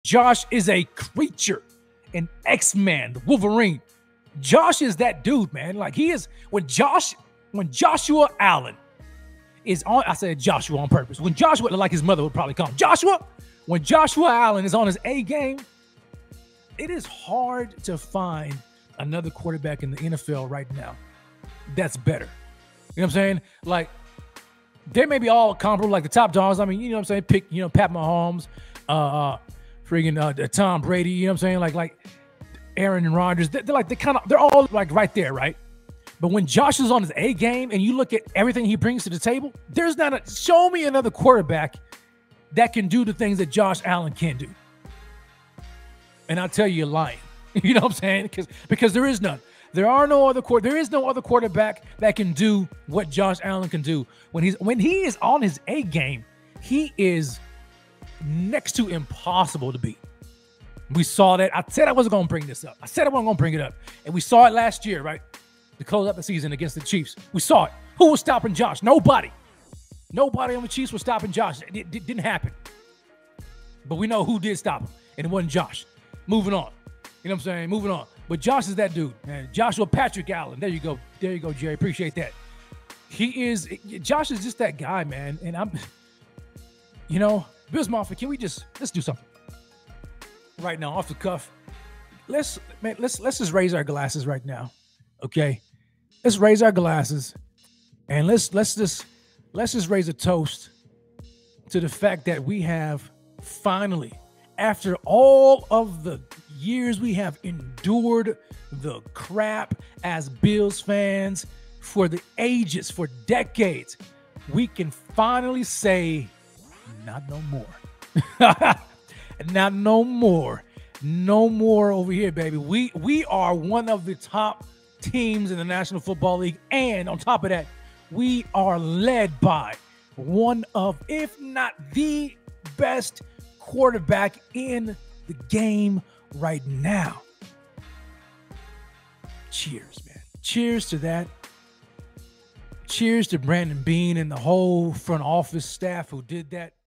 JOSH ALLEN IS A CREATURE
Bills Mafia, raise your glasses high and let's toast to the fact that we finally have an ELITE QB. Brandon Beane knew exactly what he was doing in 2018 because JOSH ALLEN IS A CREATURE! Cheers to the goodlife